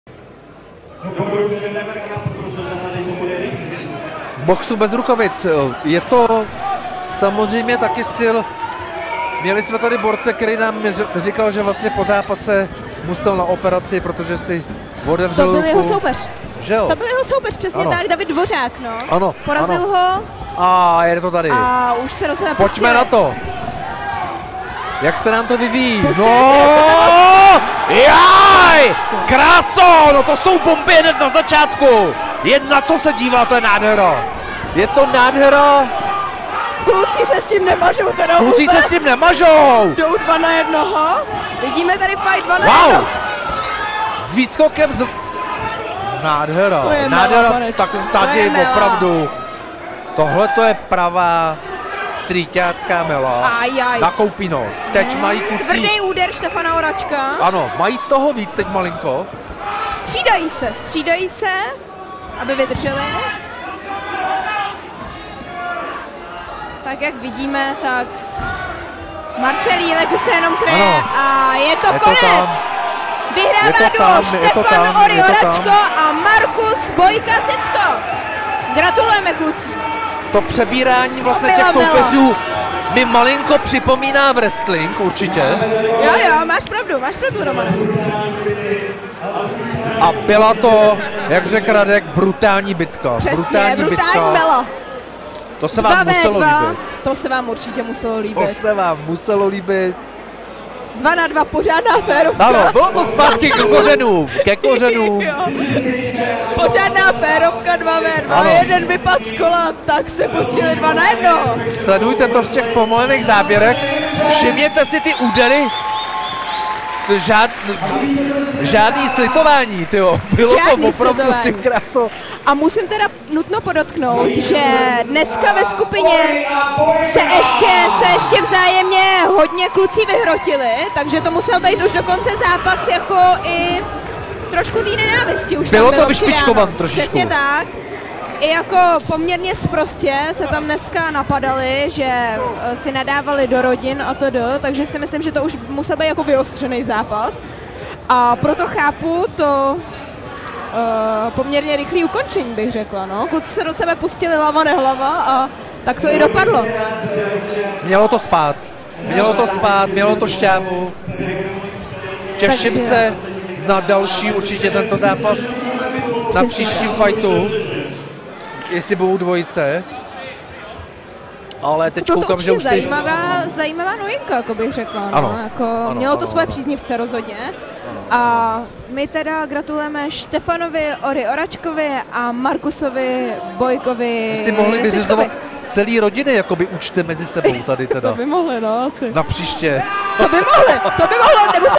Jaroslav Minařík, Marcel Jílek vs. Štefan Oračko, Marcus Cicko
Street Fight 2 vs. 2 / 3x2
Vítěz : Štefan ORAČKO, Marcus Cicko
STREET FIGHT 08
16. listopadu 2023  Depo 2015  Plzeň.